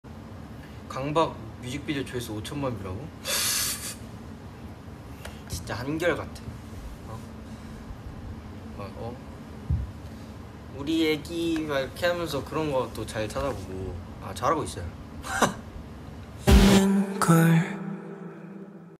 Hyunjin exposes fans for dirty mind…kpop
Not him exposing Stays like that 😂